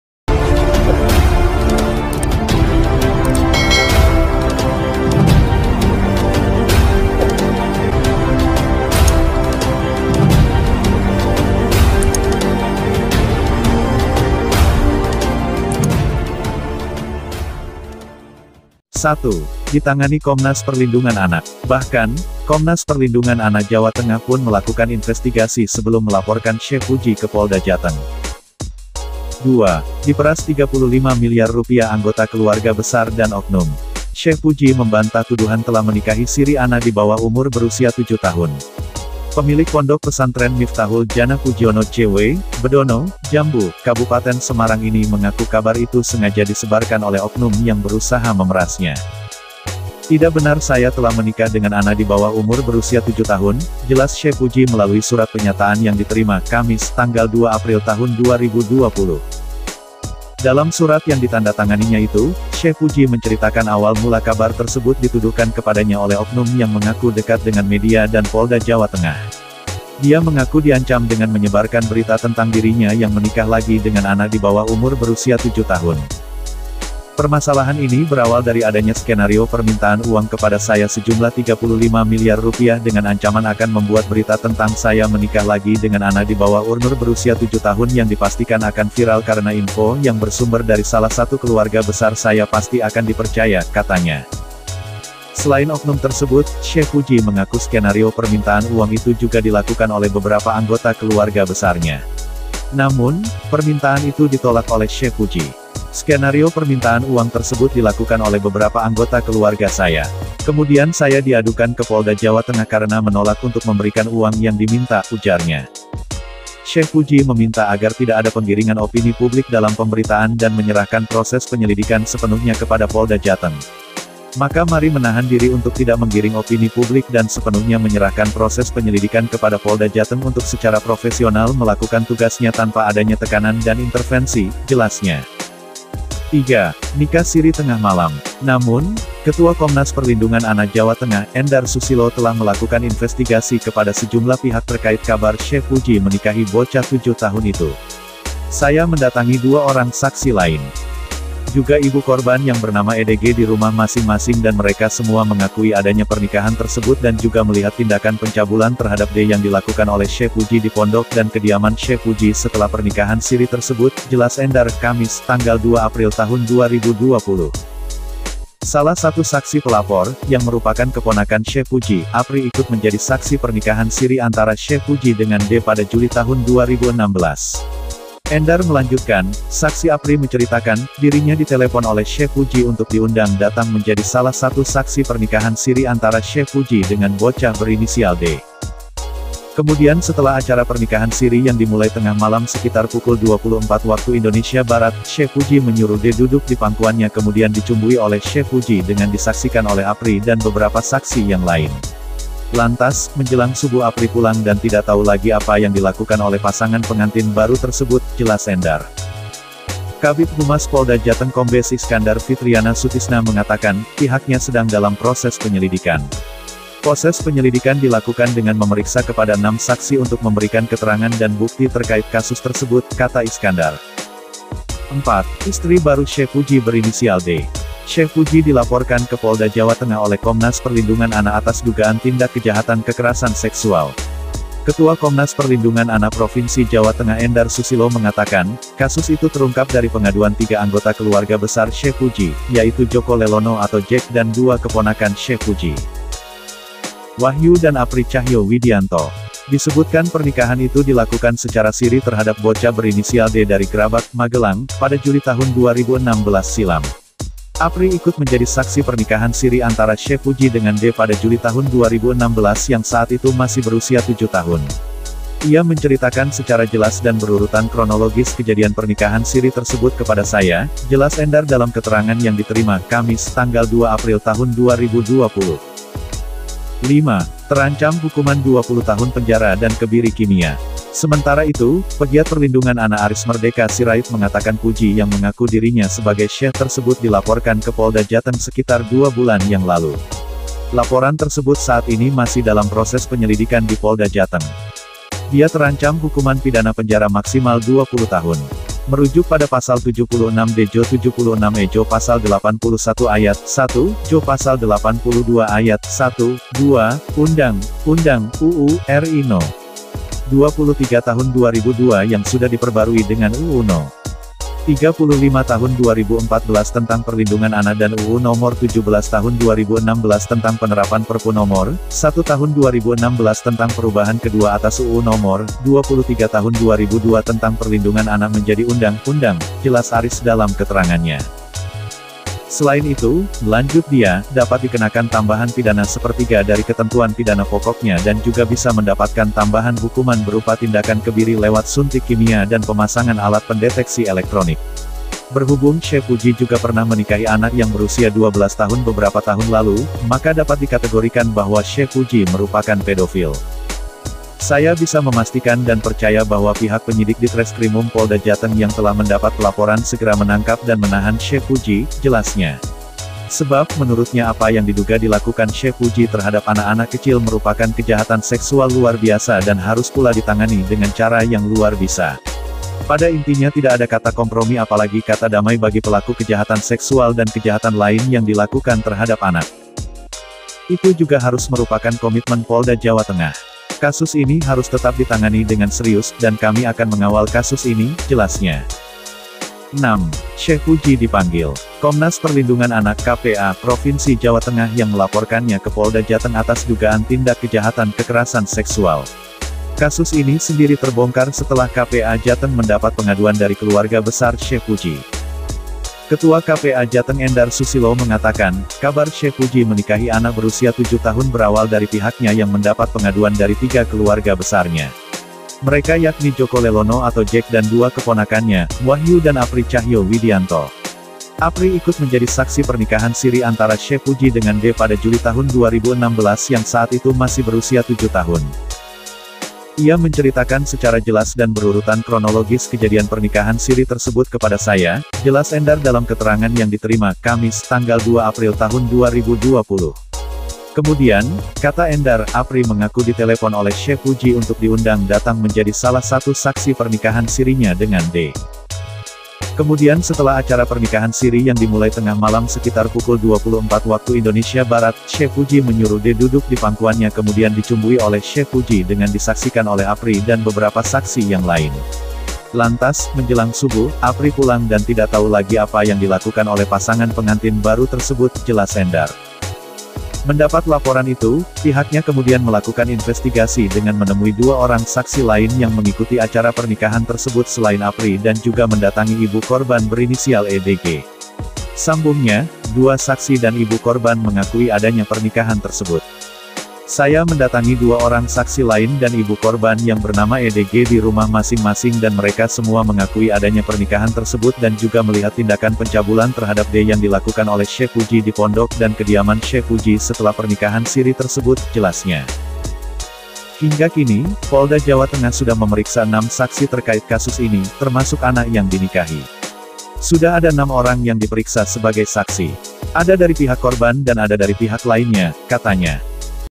6 Fakta Syekh Puji saat menikah
6 Fakta Syekh Puji Nikahi Anak 7 Tahun Diam-diam saat Tengah Malam